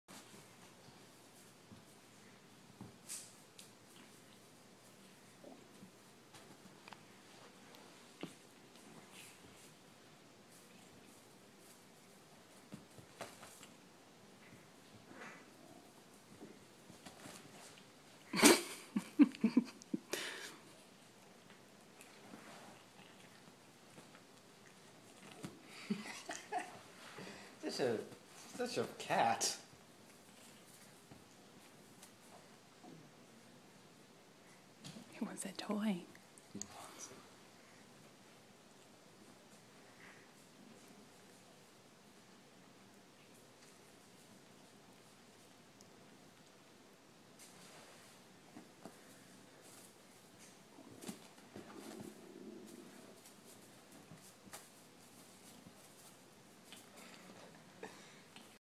Red white Persian male- MOUSE
This little guy is available to a loving forever home. He is already neutered and has his first year shots. Sold with a state of Florida health certificate validating feline testing and vaccinations. :)